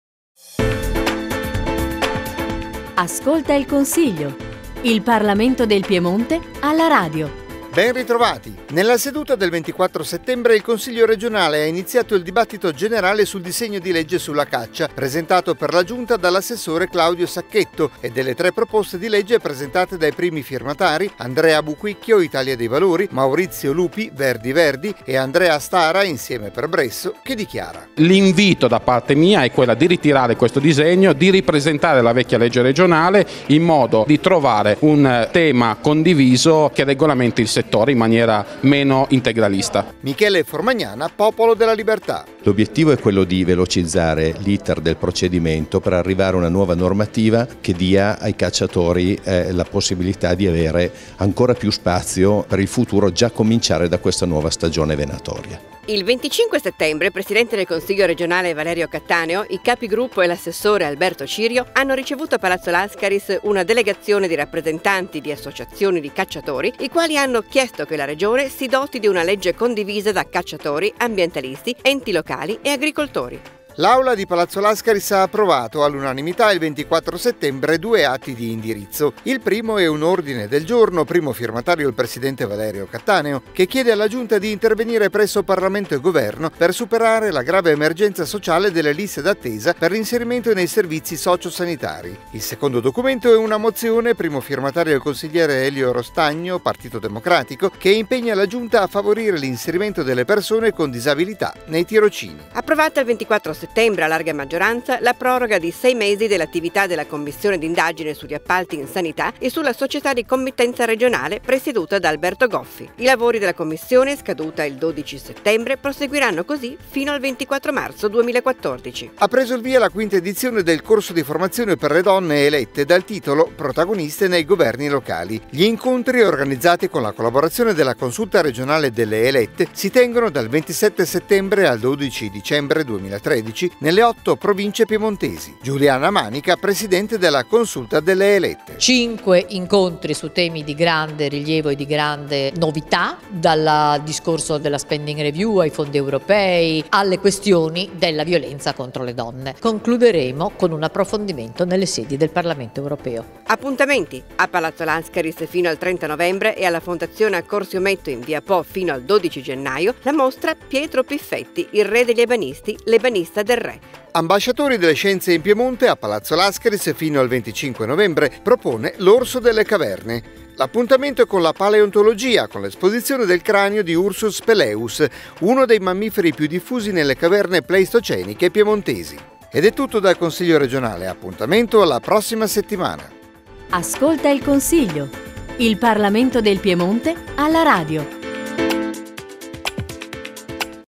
CRP Ascolta il Consiglio n.22 del 26 settembre 2013
Nella seduta del 24 settembre il Consiglio regionale ha iniziato il dibattito generale sul disegno di legge sulla caccia, presentato per la Giunta dall'assessore Claudio Sacchetto, e delle tre proposte di legge presentate dai primi firmatari Andrea Buquicchio (Idv), Maurizio Lupi (Verdi-Verdi) e Andrea Stara (Insieme per Bresso) che dichiara:
Michele Formagnana, Popolo della Libertà
Il 25 settembre, il presidente del Consiglio regionale Valerio Cattaneo, i capigruppo e l'assessore Alberto Cirio hanno ricevuto a Palazzo Lascaris una delegazione di rappresentanti di associazioni di cacciatori, i quali hanno chiesto che la Regione si doti di una legge condivisa da cacciatori, ambientalisti, enti locali e agricoltori.

L'Aula di Palazzo Lascaris ha approvato all'unanimità il 24 settembre  due atti di indirizzo. Il primo è un ordine del giorno, primo firmatario il presidente Valerio Cattaneo, che chiede alla Giunta di intervenire presso Parlamento e Governo per superare la grave emergenza sociale delle liste d'attesa per l'inserimento nei servizi socio-sanitari. Il secondo documento è una mozione, primo firmatario il consigliere Elio Rostagno (Pd), che impegna la Giunta a favorire l'inserimento delle persone con disabilità nei tirocini.

Approvata il 24 settembre, a larga maggioranza, la proroga di sei mesi dell'attività della Commissione d'indagine sugli appalti in sanità e sulla Società di committenza regionale, presieduta da Alberto Goffi. I lavori della Commissione, scaduta il 12 settembre, proseguiranno così fino al 24 marzo 2014.

Ha preso il via la quinta edizione del corso di formazione per le donne elette dal titolo "Protagoniste nei governi locali". Gli incontri, organizzati con la collaborazione della Consulta regionale delle Elette, si tengono dal 27 settembre al 12 dicembre 2013 nelle otto province piemontesi.
Giuliana Manica, presidente della Consulta delle Elette

Appuntamenti
* a Palazzo Lascaris fino al 30 novembre e alla Fondazione Accorsi-Ometto in via Po fino al 12 gennaio, la mostra "Pietro Piffetti. Il re degli ebanisti, l'ebanista del Re". Si tratta della prima rassegna monografica dedicata al raffinato artigiano intagliatore nel legno, nato a Torino nel 1700 e morto nella stessa città nel 1777.
* Ambasciatori delle scienze in Piemonte" a Palazzo Lascaris fino al 25 novembre propone "L'orso delle caverne". L'appuntamento è con la Paleontologia con l'esposizione del cranio di Ursus spelaeus, uno dei mammiferi più diffusi nelle caverne pleistoceniche piemontesi.